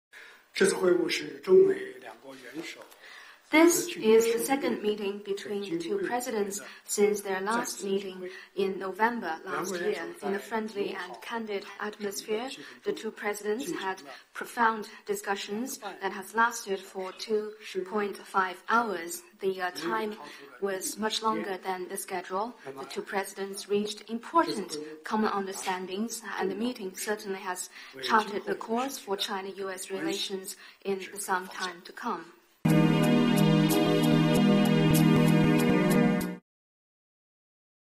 Cuộc gặp giữa Chủ tịch Tập và tổng thống Trump đạt được sự đồng thuận "quan trọng"
Chủ tịch Trung Quốc Tập Cận Bình và tổng thống Mỹ Donald Trump đã đạt được sự đồng thuận "quan trọng" trong bữa tối làm việc vừa kết thúc bên lề Hội nghị thượng đỉnh G20 tại Buenos Aires, Argentina, Bộ trưởng Ngoại giao Vương Nghị cho biết tối ngày 1/12.